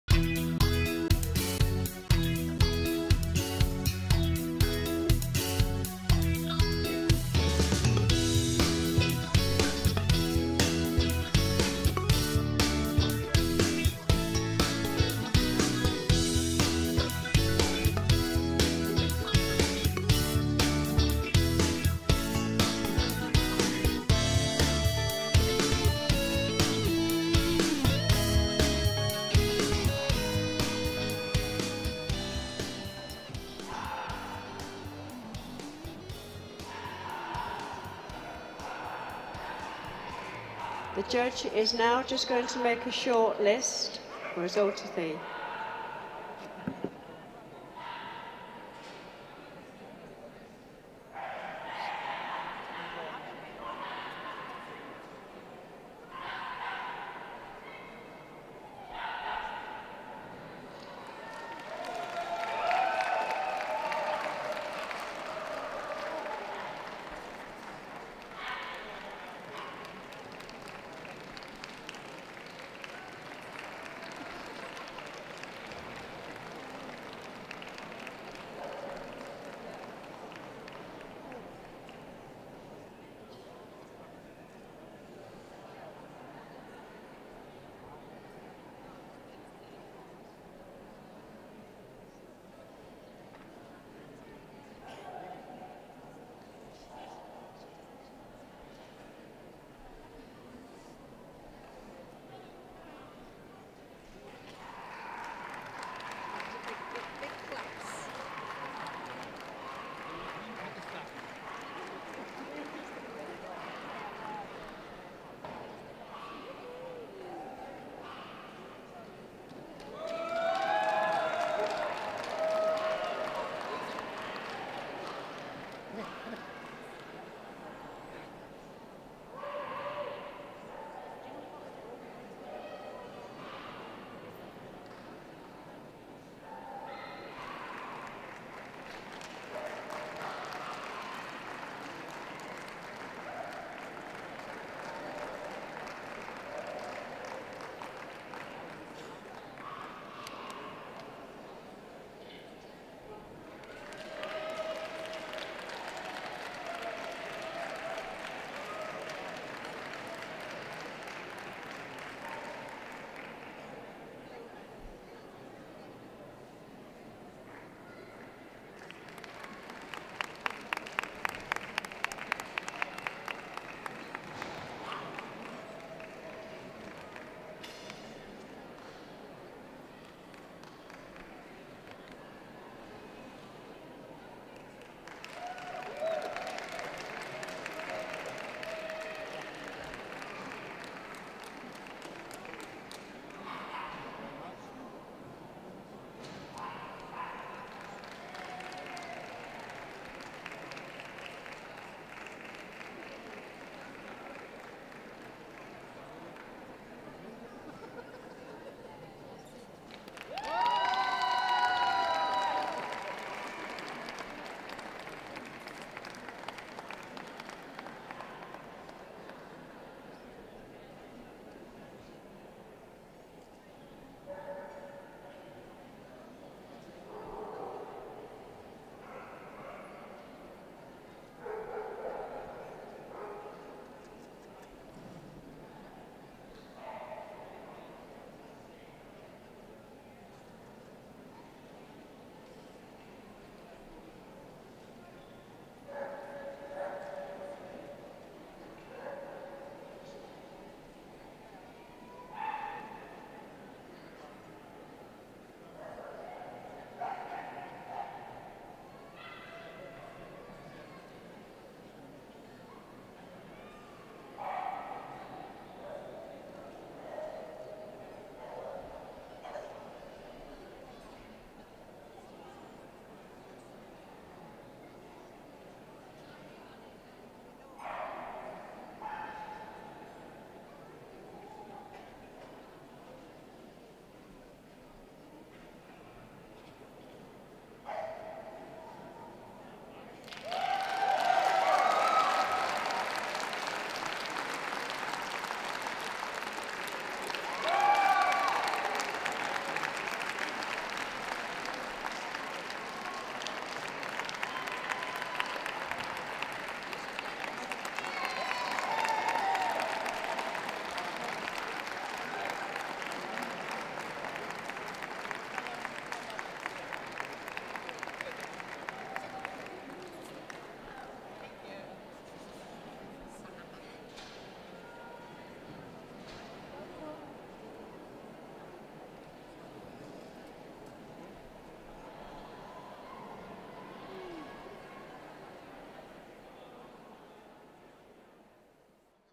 National Terrier 2017 - Best in Show Highlights
Best in show at National Terrier championship dog show 2017 under judge Roger Thomas was the Lakeland who also won the group at Crufts, Ch Saredon Enigma, bred and handled by John Averis and owned by Johann Schembri from Malta and Tony Barker.
Reserve Best in Show went to the Welsh - Monaline Lagoon Lee with Kerry Blue Ch Torum's Cairo Fred in third and fourth going to Ch Honeymist Posh Dreamz the Bedlington.
Roger also judged Best Puppy in Show, watch that here too: